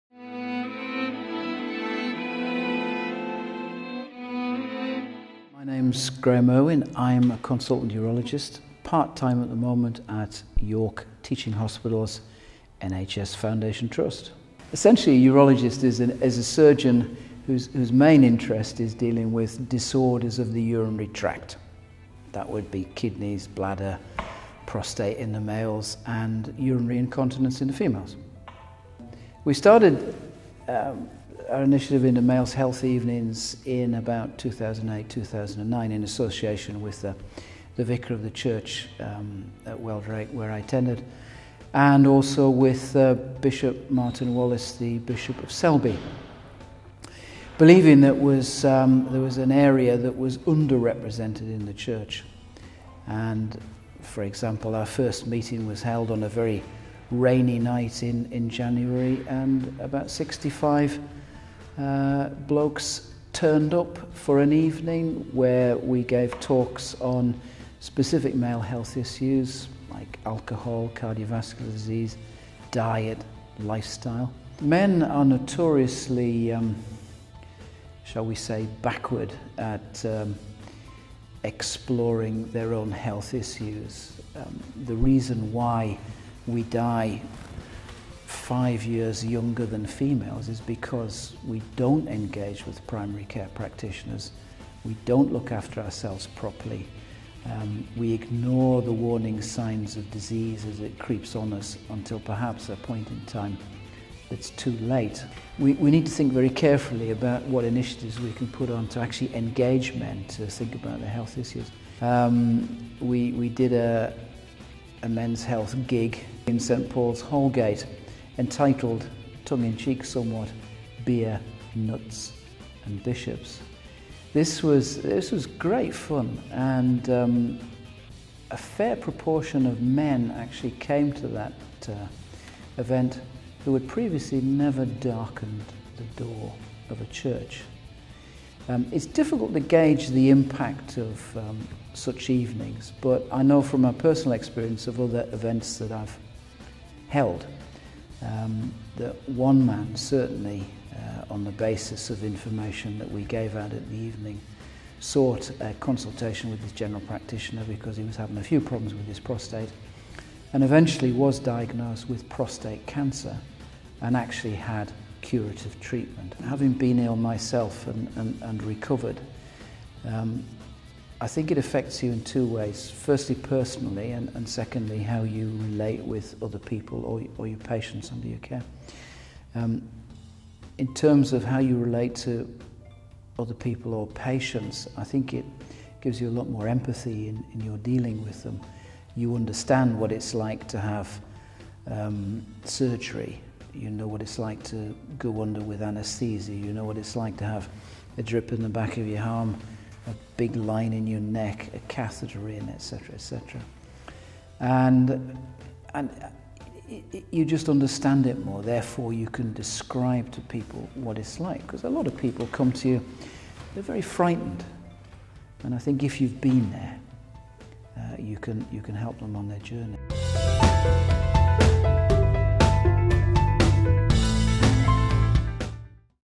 Faith Story Graeme Urwin.mp4
Graeme Urwin is a consultant urologist at York Teaching Hospital NHS Foundation Trust. Graeme features as part of the Archbishop of York's Faith Stories Book published in February 2013